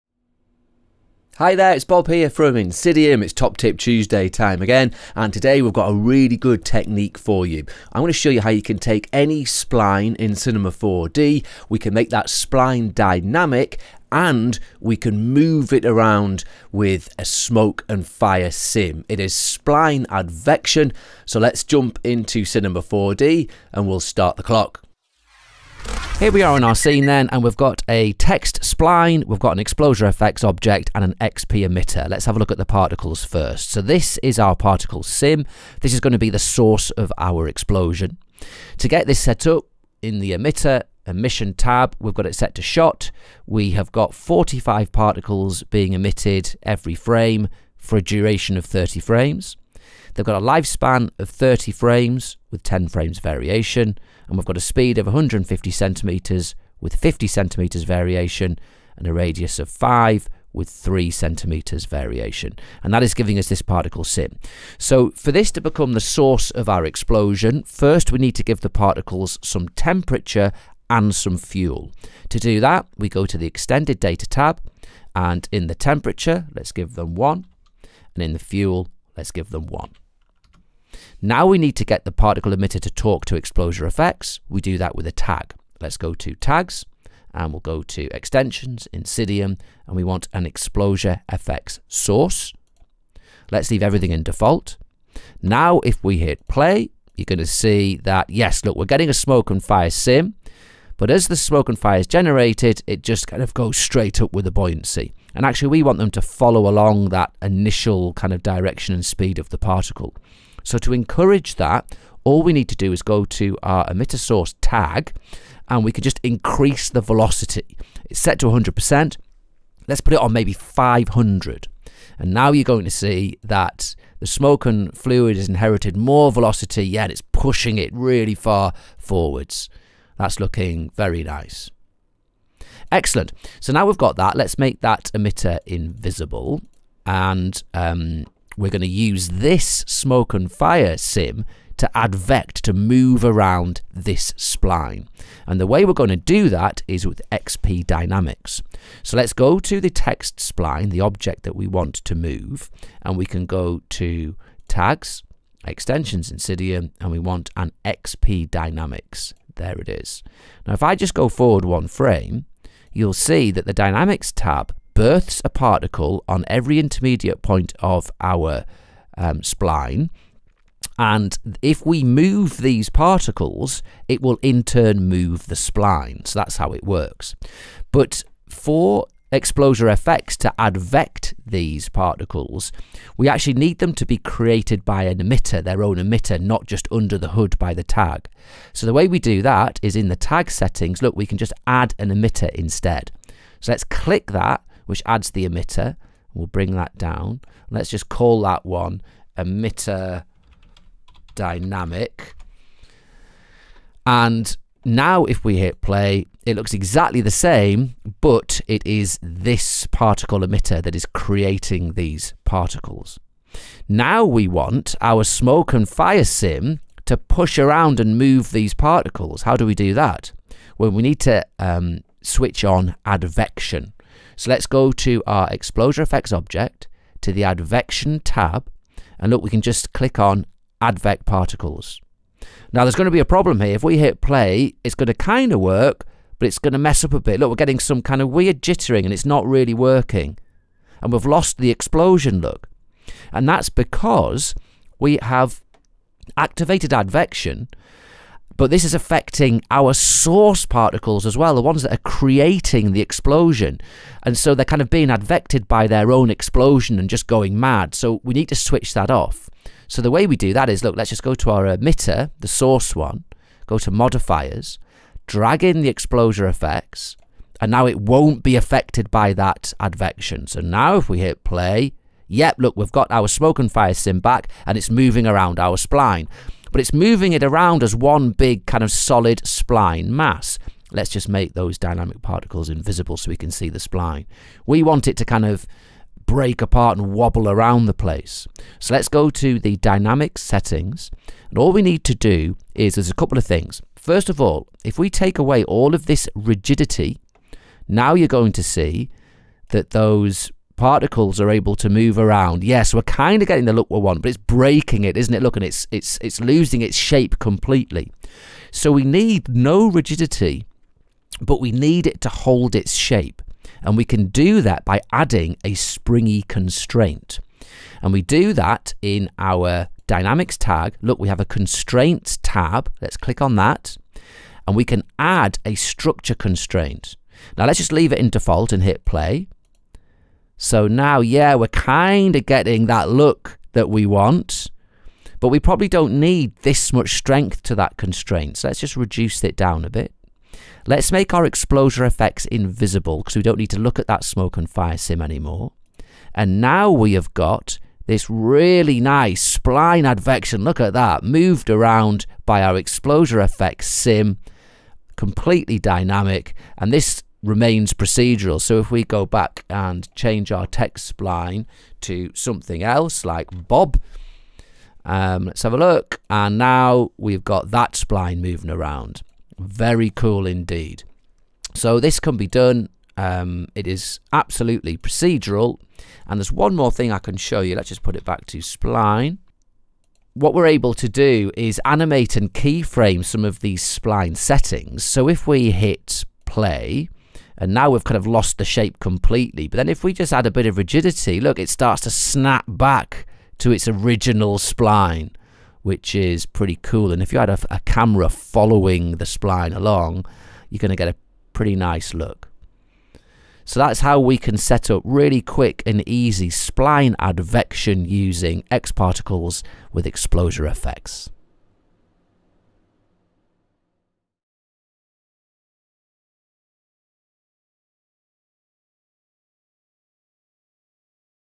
Top Tip Tuesday - Spline Advection
This week's Top Top Tuesday features an xpExplosiaFX sim, in advection mode, being used in conjunction with xpDynamics, to generate particles on a source spline.

Dynamic settings and constraints will be adjusted to give a smooth spline deformation.